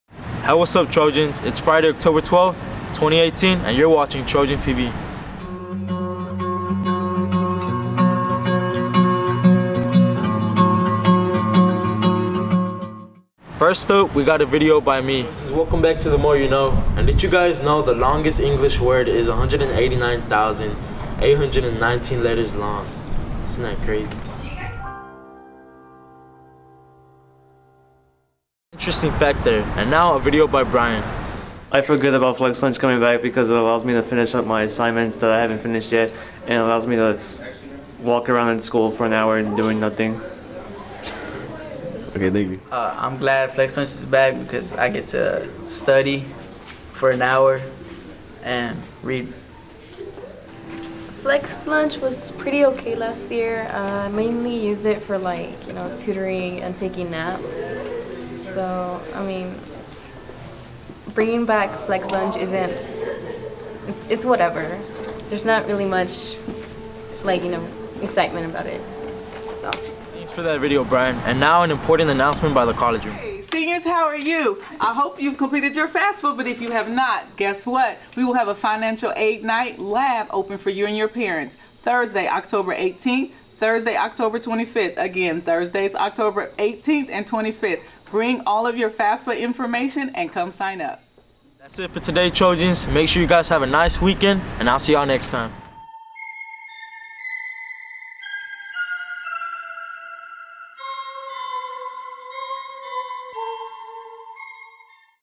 Trojan TV 10-12-18
Today's show has the more you know, a video about flex lunch and an important announcement about financial aid.